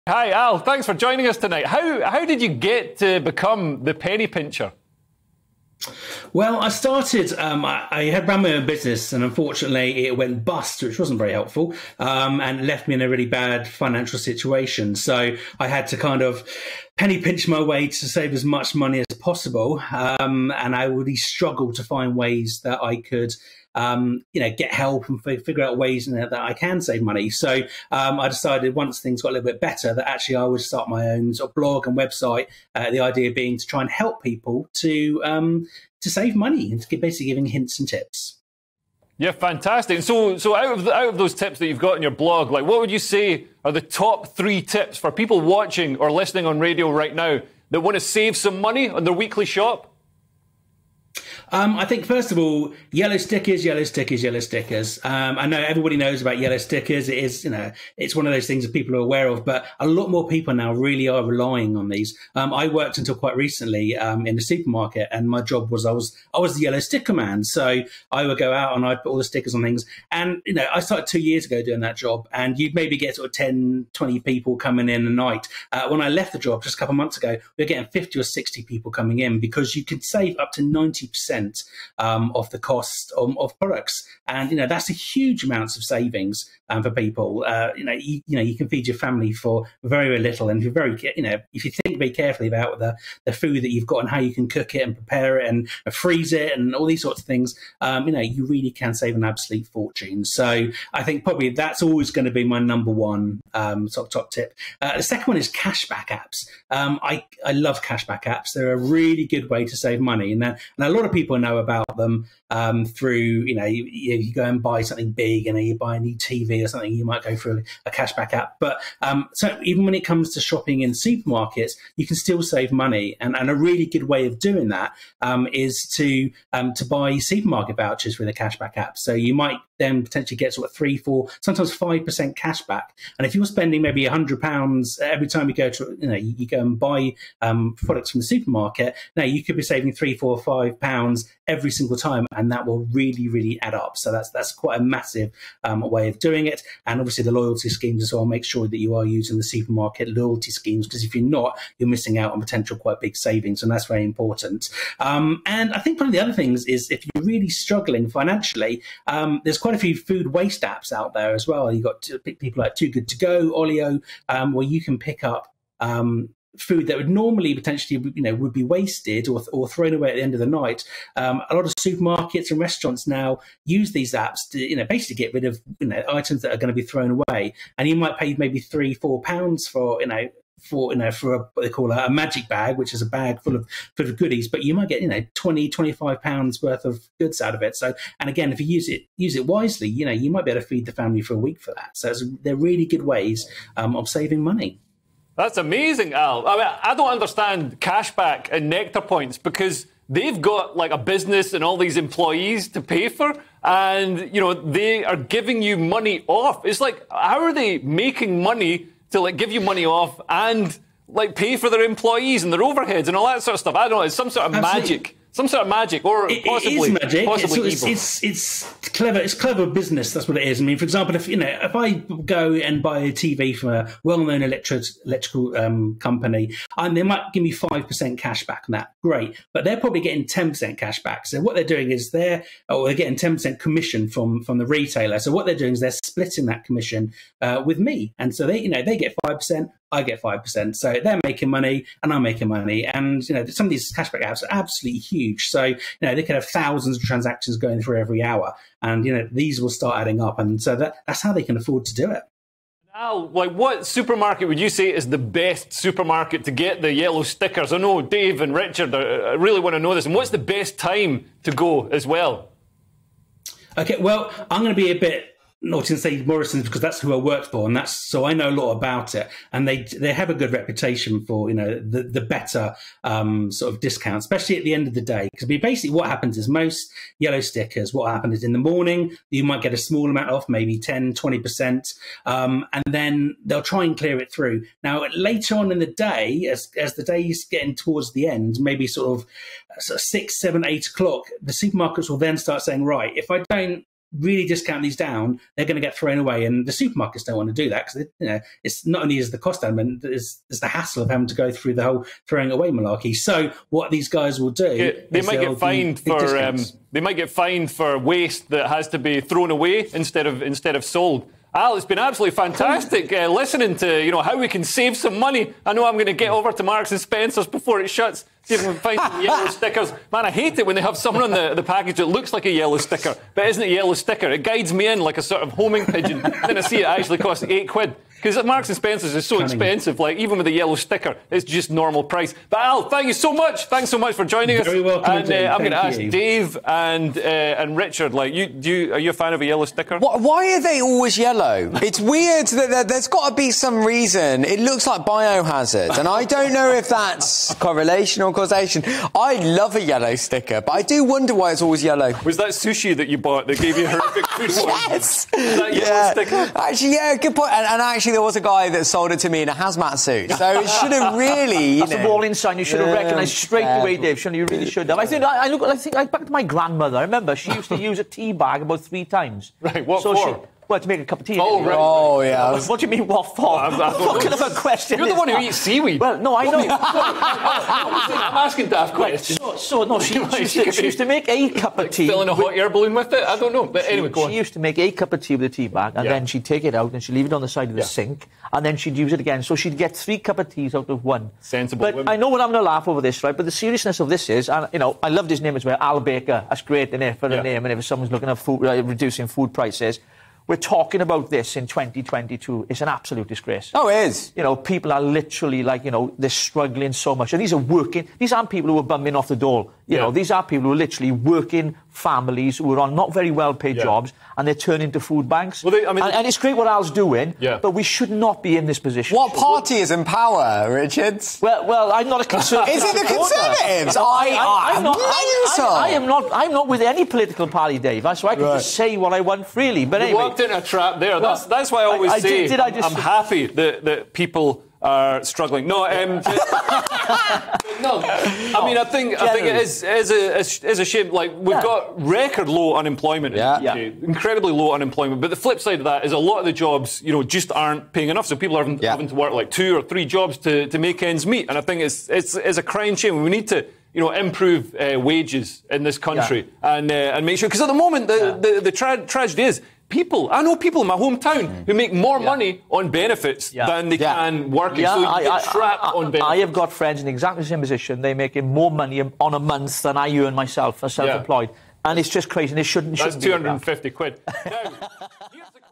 The Penny Pincher Al Baker gives his top tips for saving money on food
'You can really save an absolute fortune buying products with the reduced yellow sticker. That will always be my number one top tip.'

Al Baker, The Penny Pincher, gives Leo Kearse his best tips for saving as much money as possible on food.